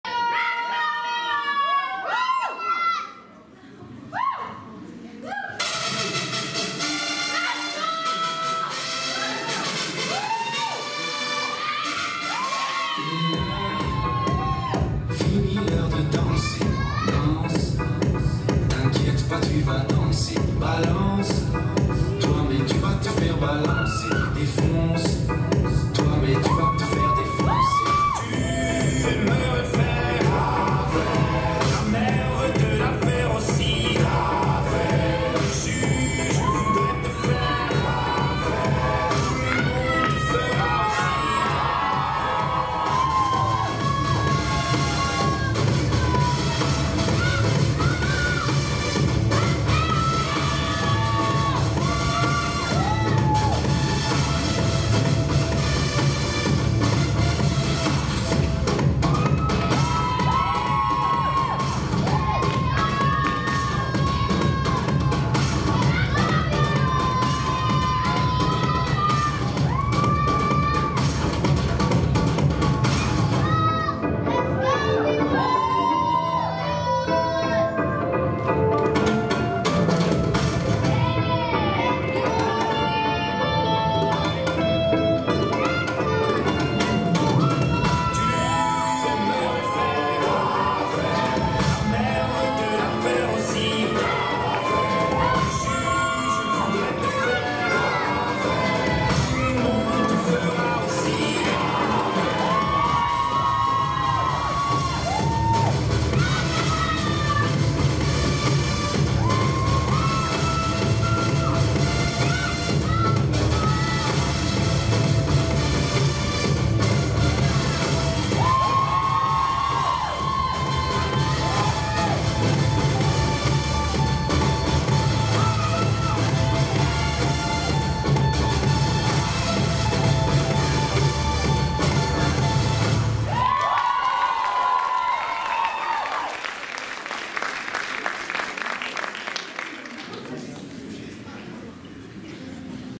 23-24 Qualifier BE - B One (Danzateljee)
*B One  from Danzateljee in Berchem, BE
*First place Hip hop Teens Group at the qualifier in Houthalen-Helchteren on the 6th of April.
*Choreography by Rani Rillaert En Emely Van Nespen